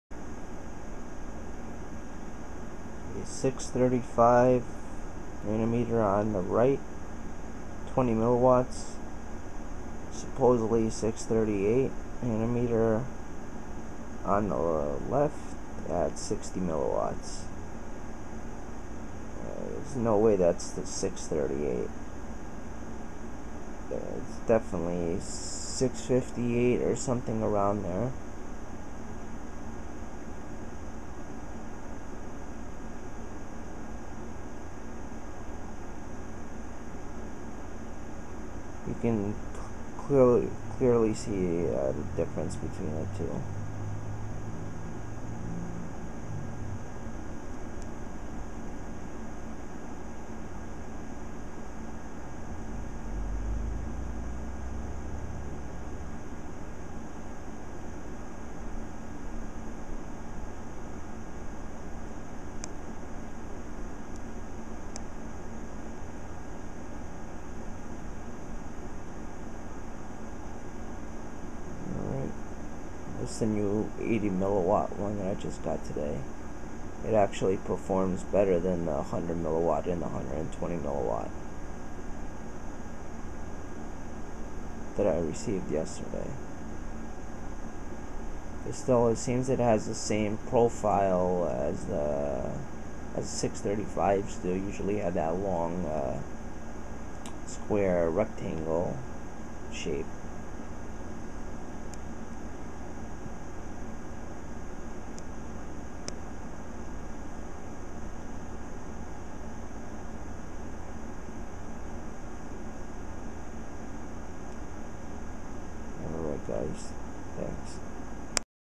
660nm vs 635nm
660nm at around 60mW and on the Right is a 21 635nm.

You can see that the 638nm is not that.

Here is A video of some supposedly 638nm diodes from ebay, But when i tested them The are 658nm.

But even though they dint come out to be 638nm the dot on them does not have wings like other 660nm